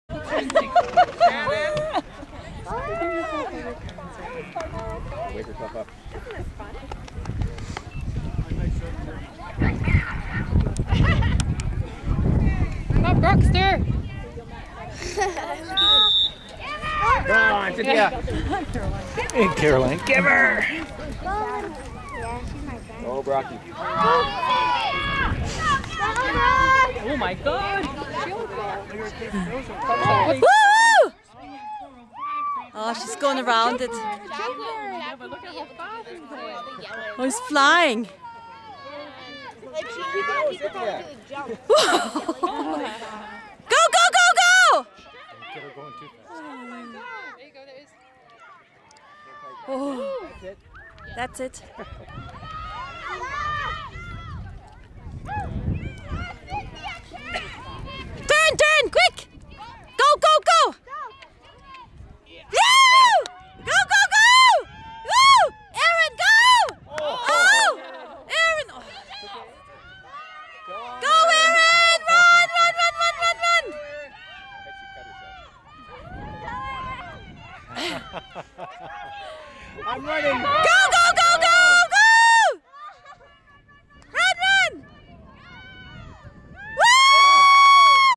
Willow Grove Magic in the Air 2008 "6 legged race"
Cynthia and Erin in the Horse and Human Relay at Willow Grove 2008